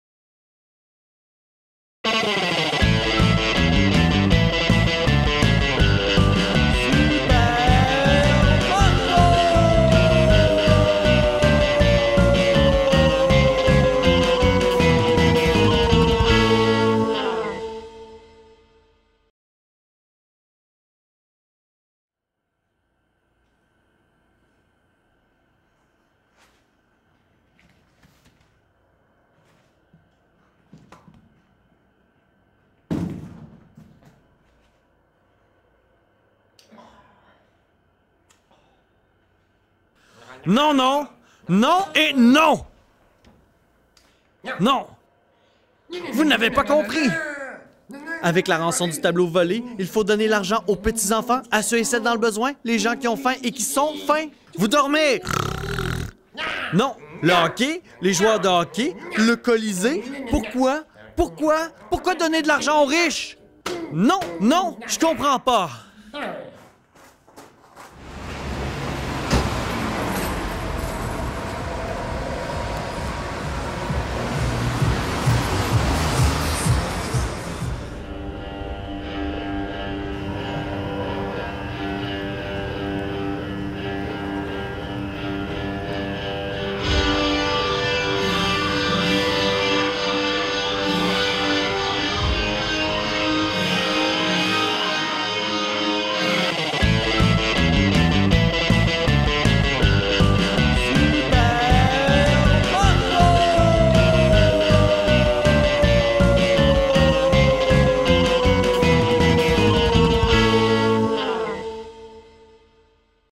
Super Marteau - épisode 2
Une toile de Van Gogh volée en Égypte.
Un super-héros (?) qui exige une rançon.
La population qui choisit à qui doit aller l'argent.